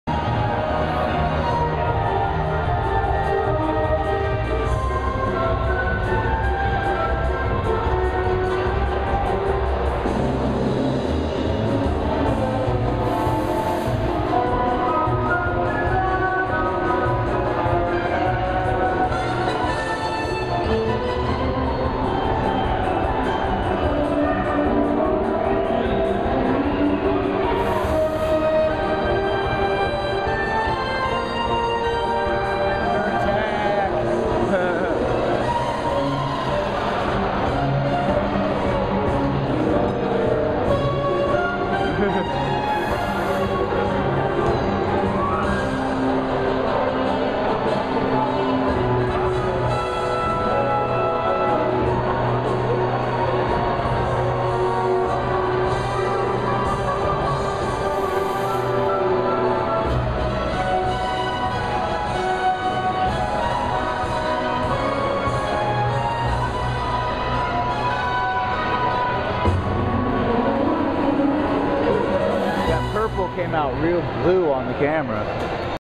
Lazer Show @ the Planet Holywood
Some lazer show my bro and I wandered into after hours of wandering the Vegas strip. We thought we found a good bench to finish our smokes and rest our feet, then poof lazers. I think this was inside the Planet Holywood